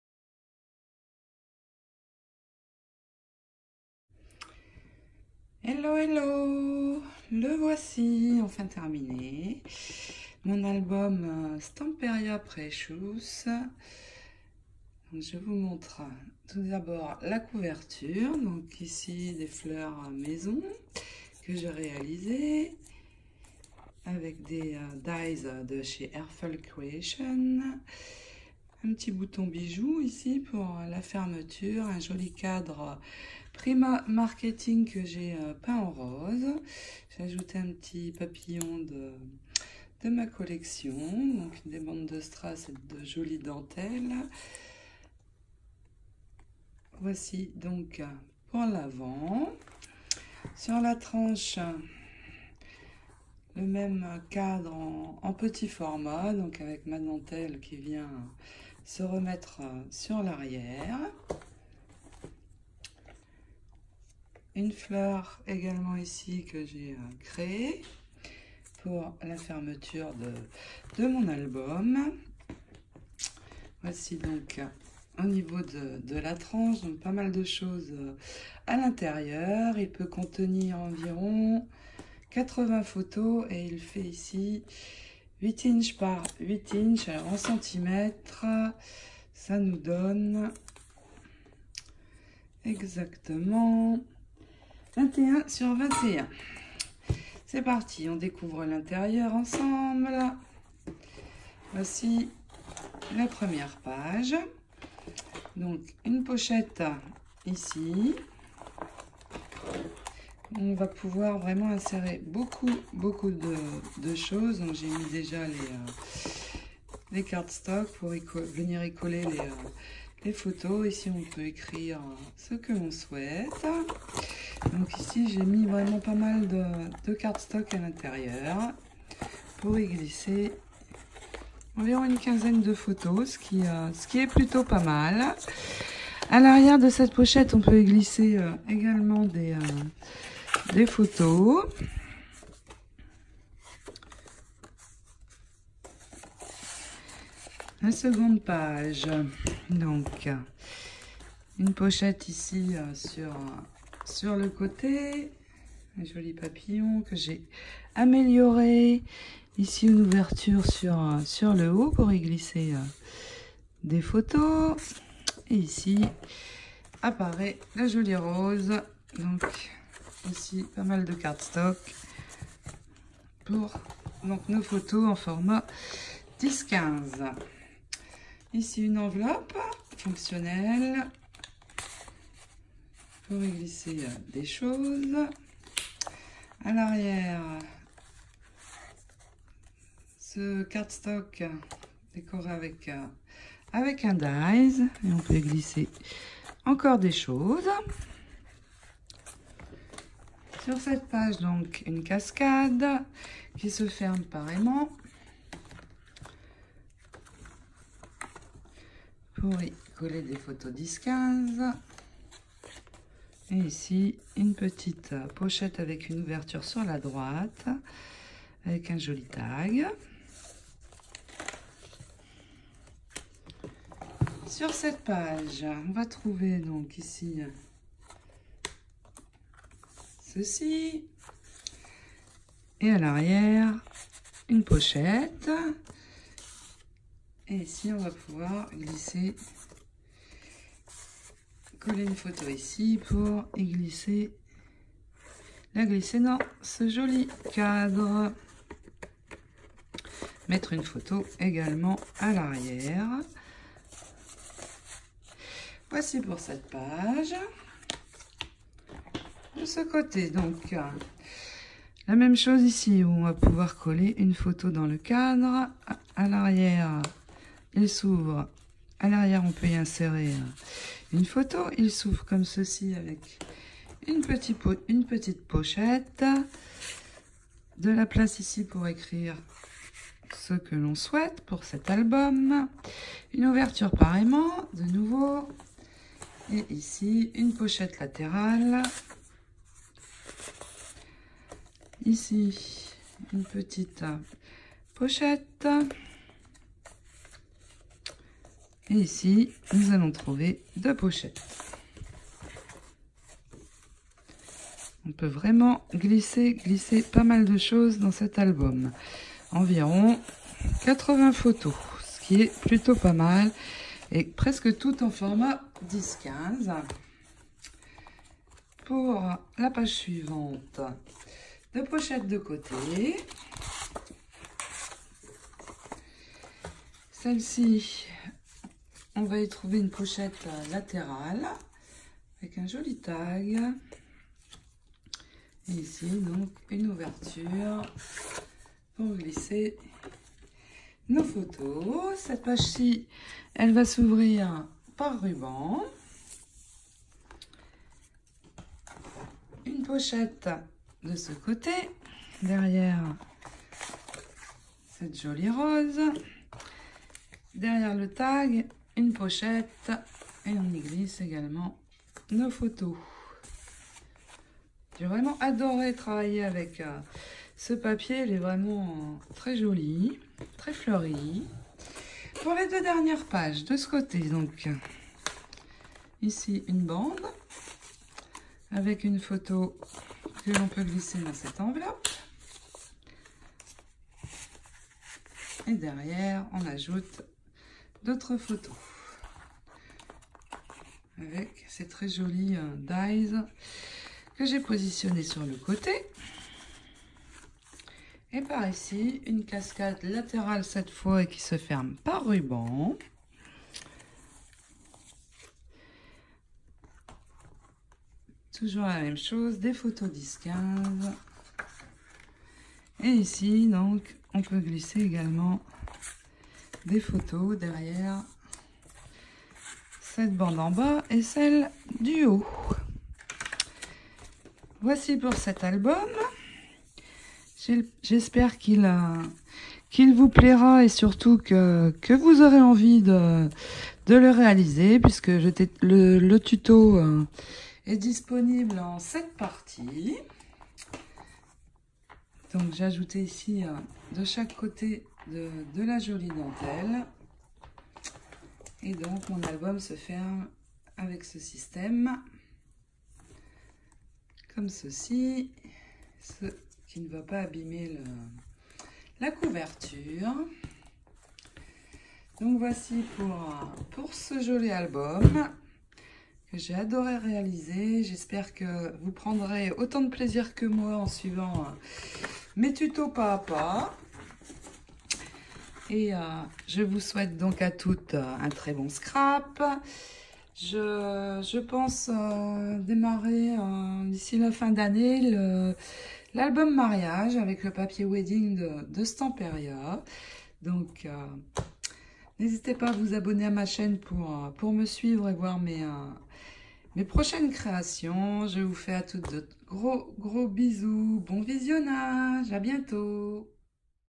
Présentation album stamperia précious :21x21 -80 photos
Album réalisé avec 2 blocs de papiers stamperia précious pouvant contenir environ 80 photos   avec tutoriels disponibles en 7 parties 😉
Bisous bisous 😘
Helen scrap